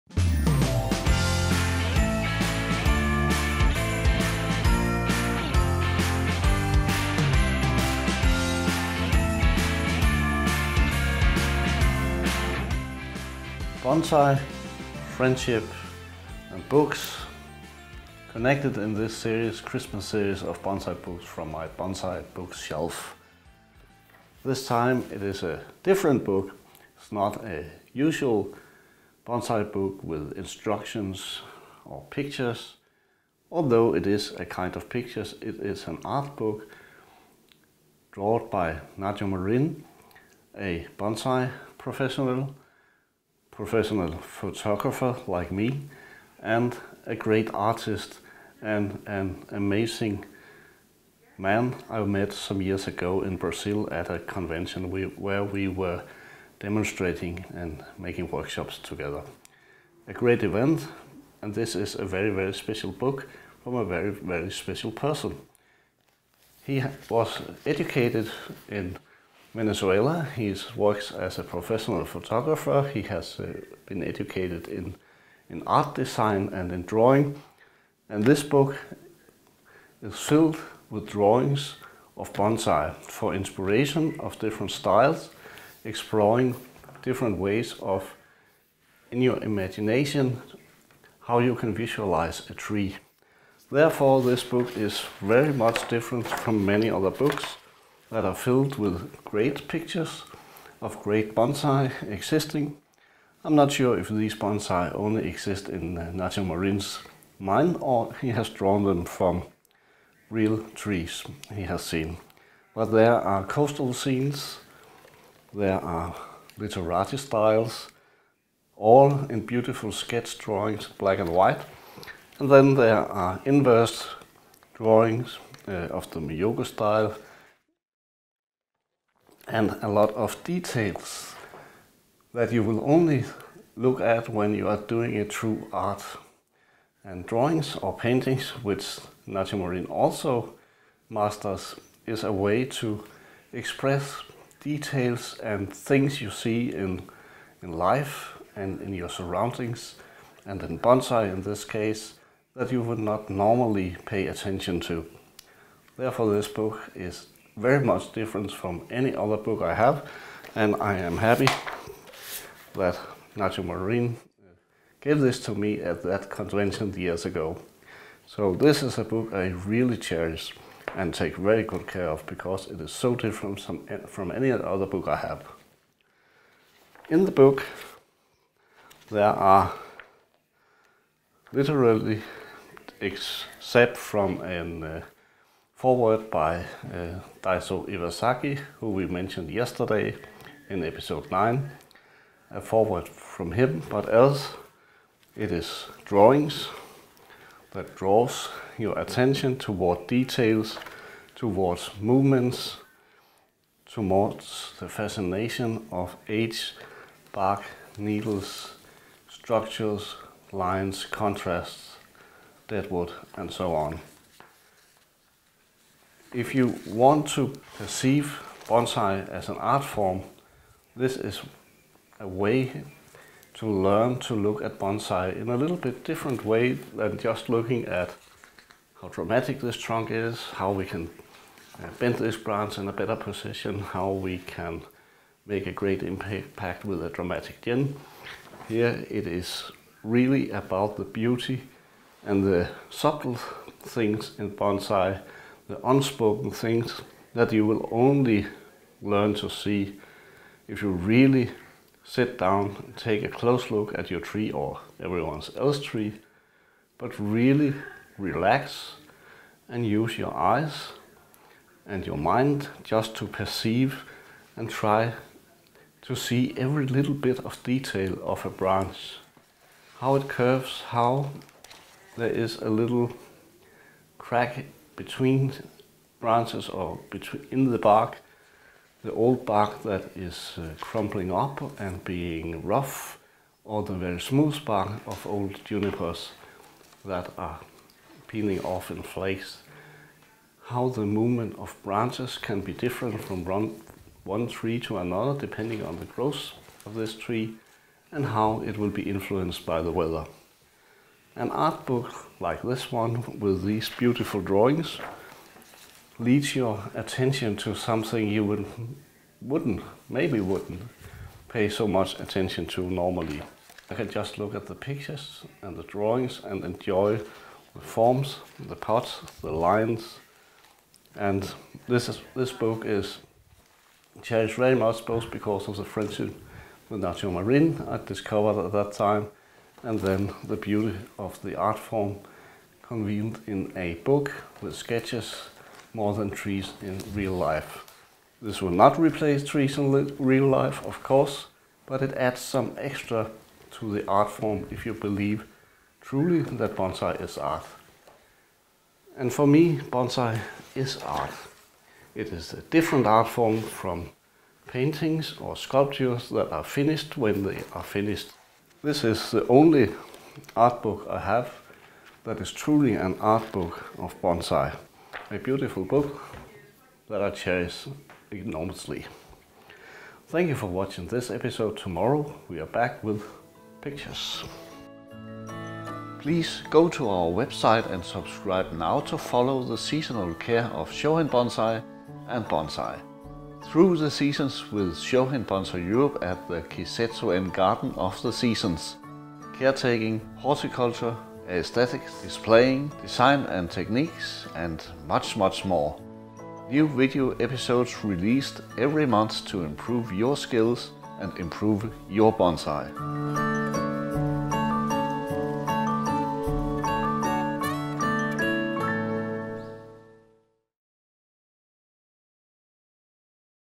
Bonsai Christmas Calendar episode 10 - Nacho Marin
A bonsai art book by Nacho Marin from Venezuela.
A rare book with bonsai drawings and high quality. Watch the review in the 10th episode of the Kisetsu-en Bonsai Christmas Calendar.
Morten Albek - Shohin Bonsai Europe.